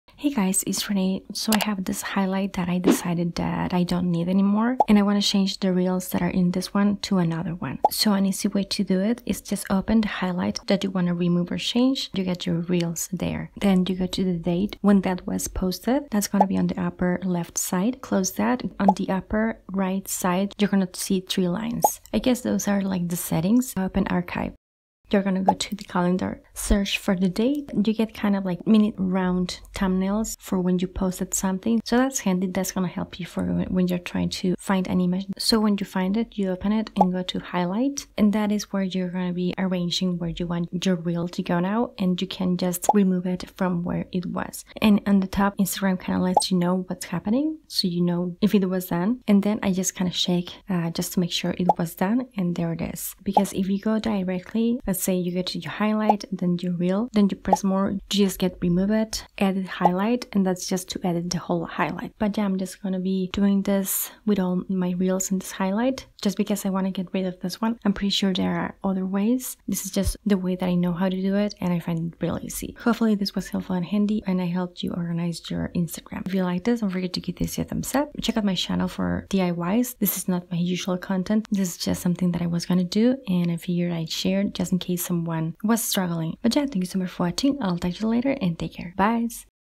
How To Move Reels To Different Highlights
Hi! I hope you like this Move Reels From One Highlight To Another DIY!